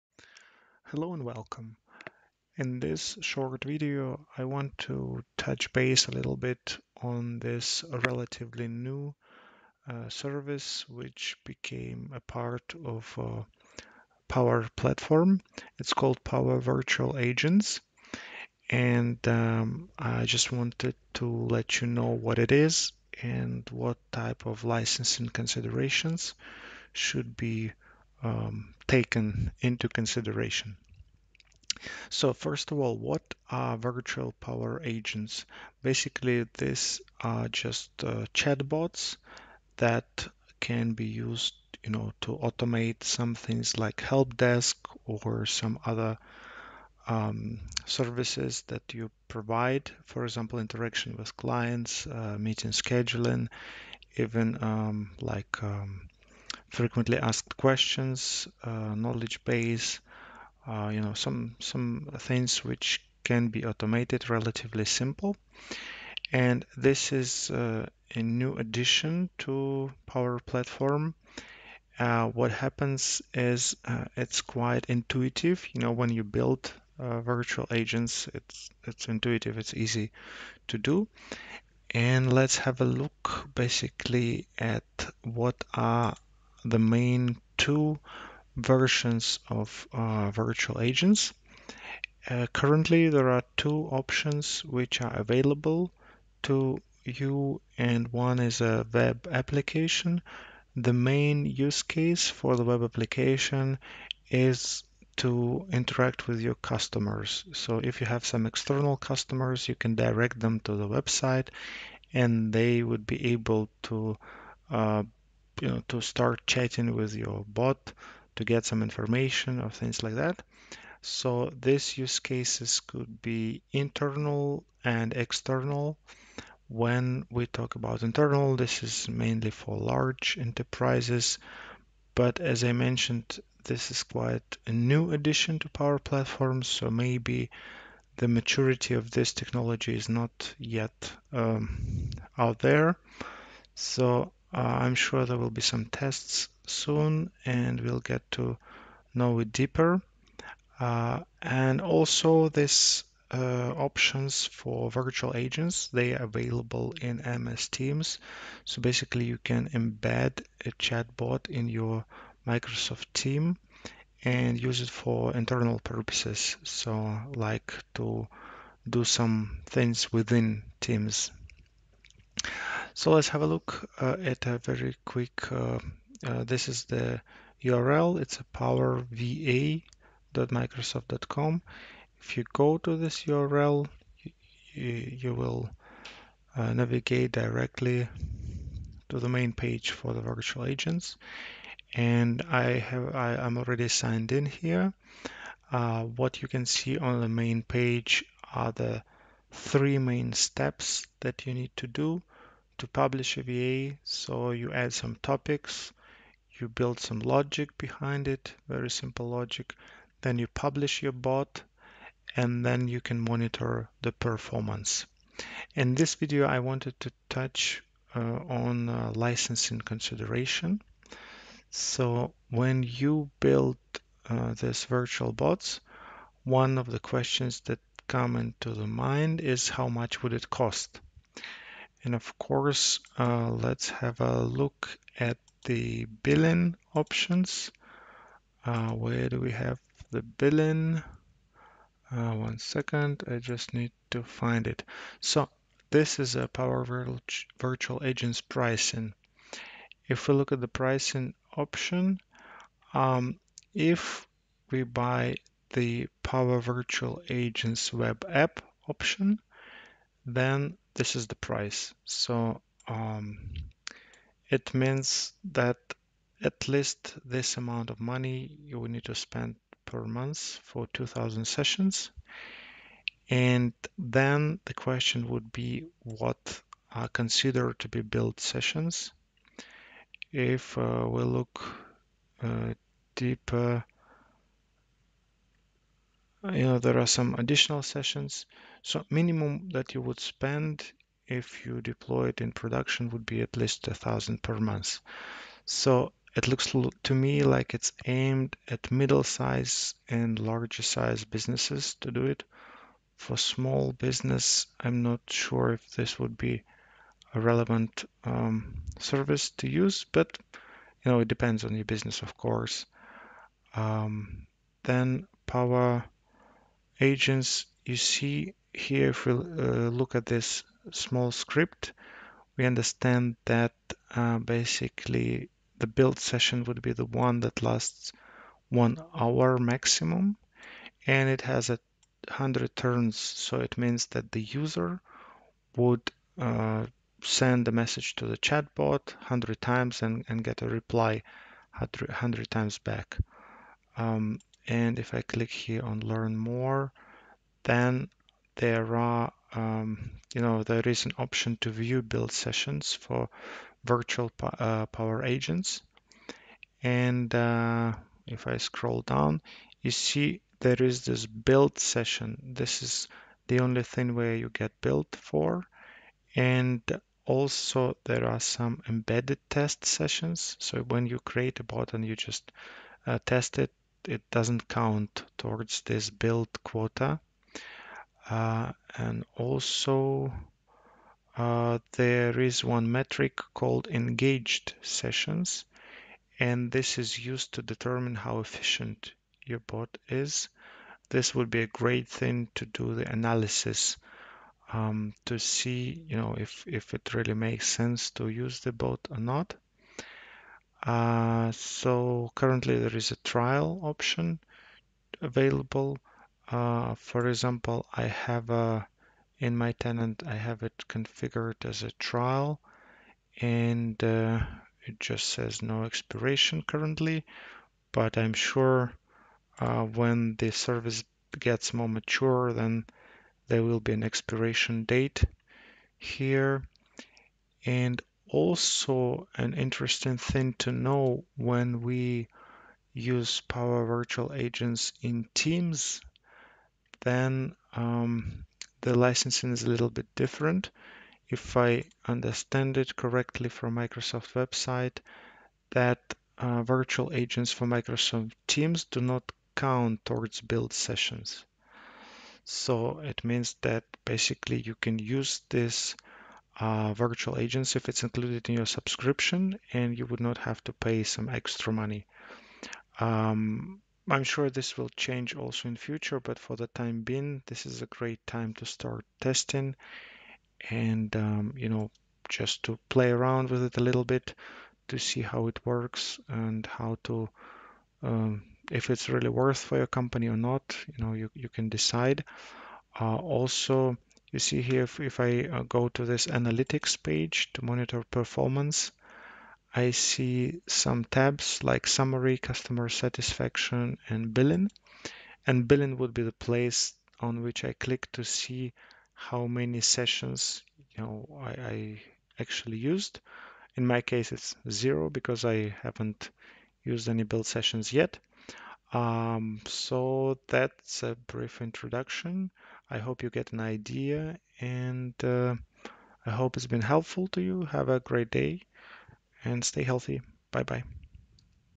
Power Virtual Agents - introduction to licensing
Power Virtual Agents are easy to create chatbots, how to get started and how much would it cost you